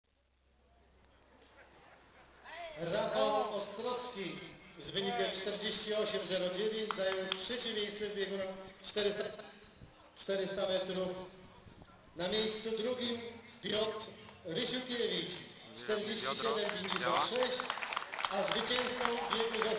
Tomasz Majewski - 20.30 Kędzierzyn-Koźle 2008-06-06
Występ Tomasza Majewskiego na Mitingu pucharu Polski i otwarcia stadionu " Kuźniczka " w K-Koźlu Kugelstoss ядро